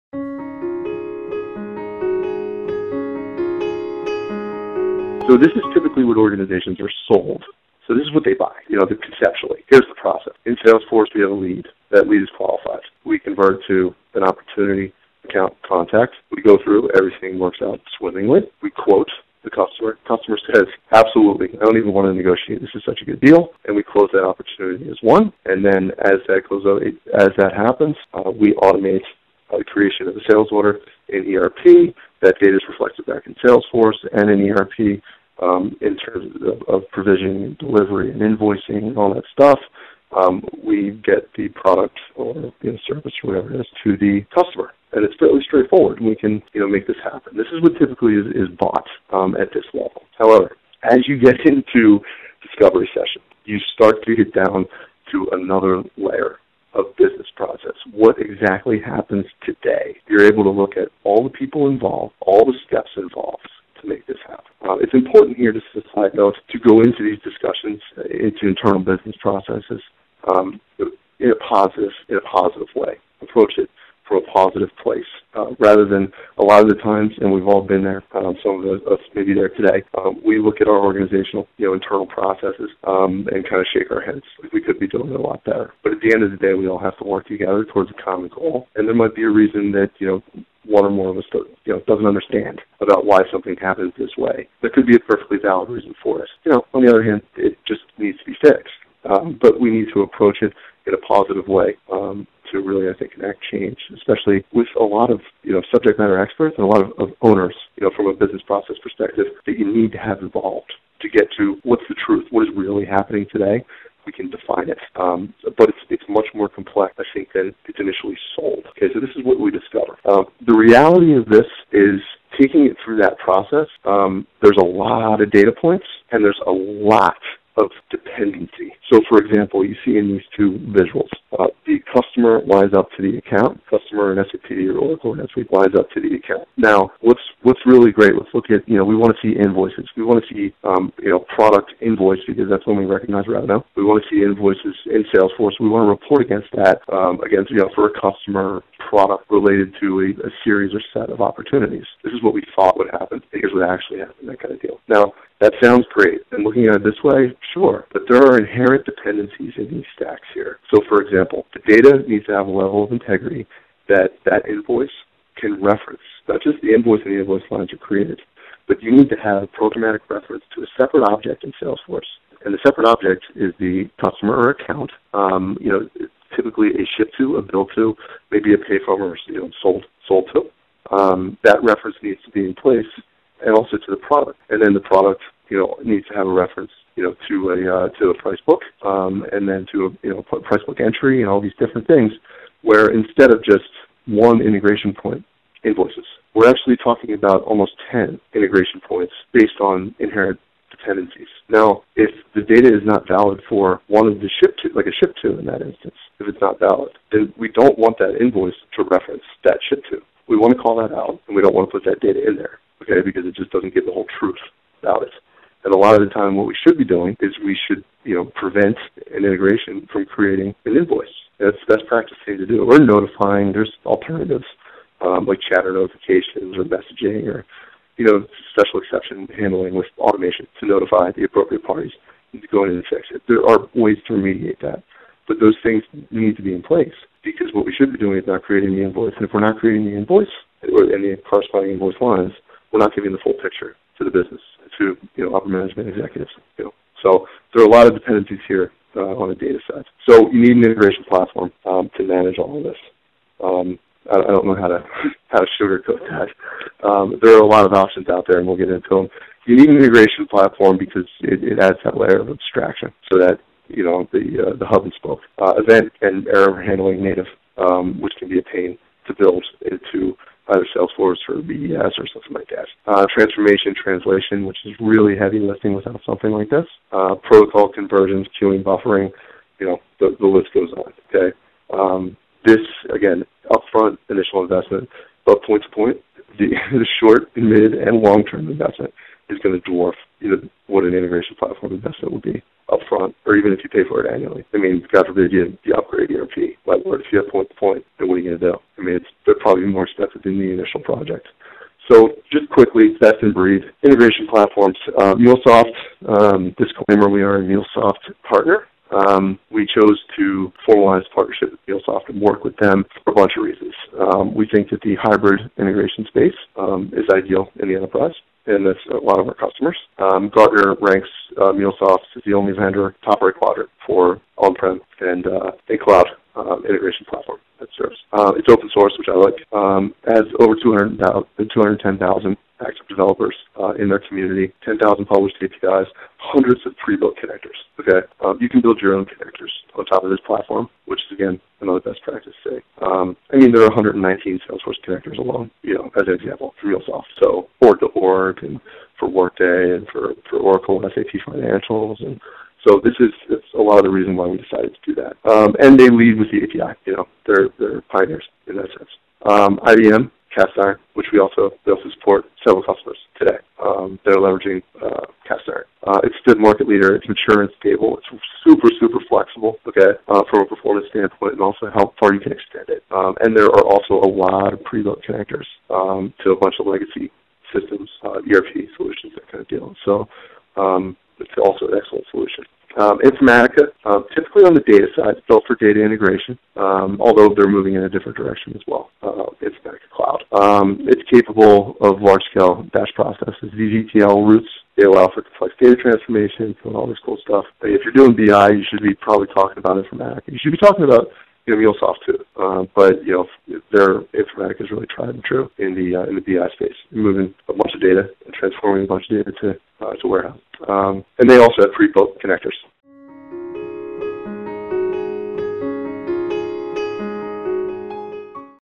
Salesforce Integration Best Practices
Watch this video to learn how you can build a unified, scalable, and maintenance-free set of business apps.